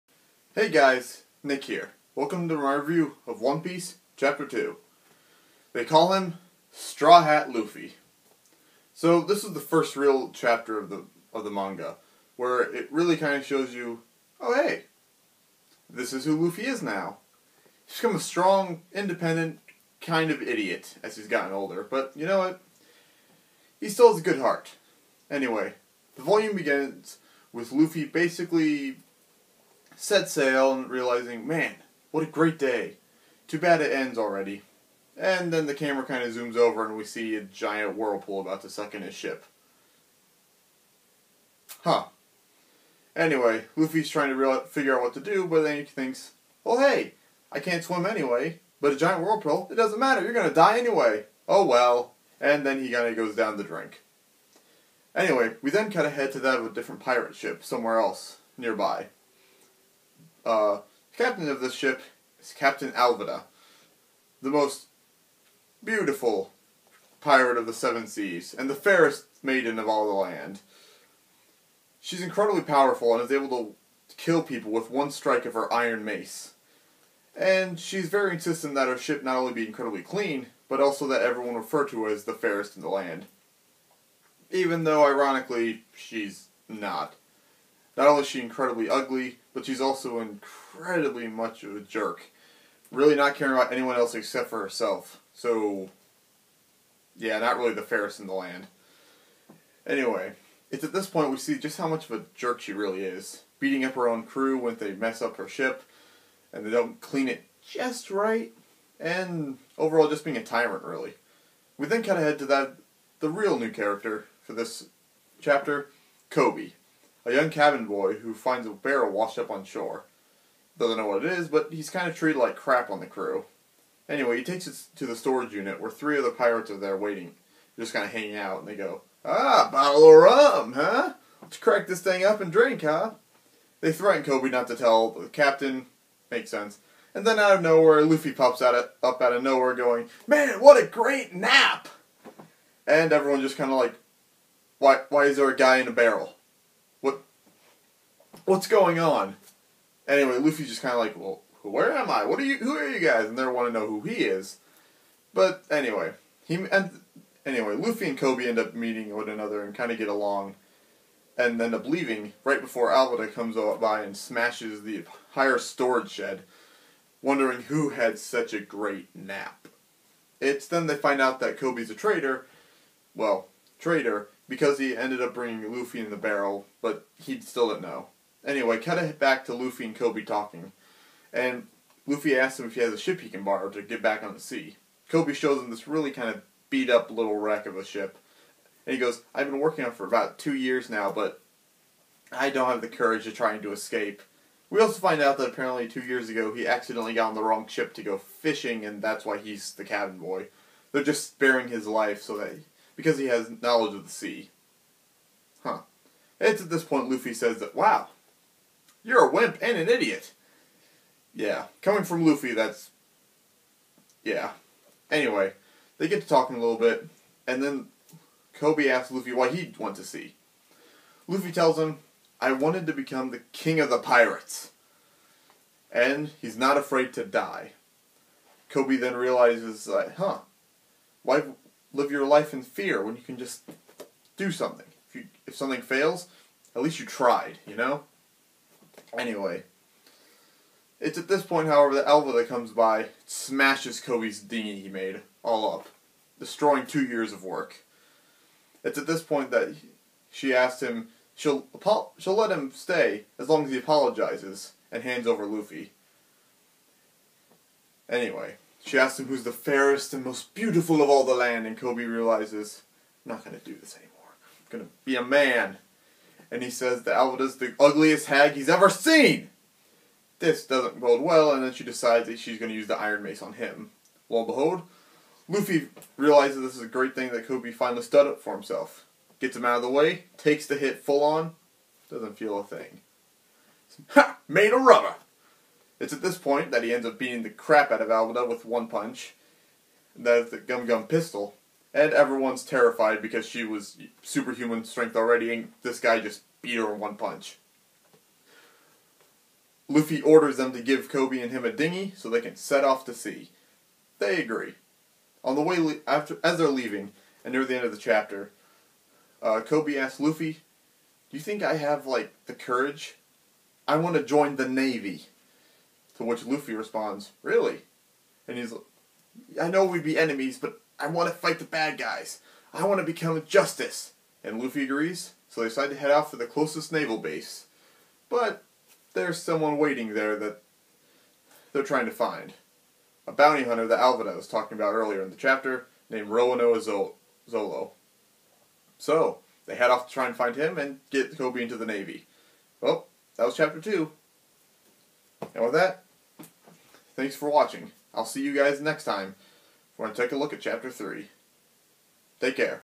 One Piece Review Chapter 002 "They Call him Straw Hat Luffy"
Chapter 2 is here and we meet up with the younf cabin boy Coby and the evil Captain Alvida in today's Chapter!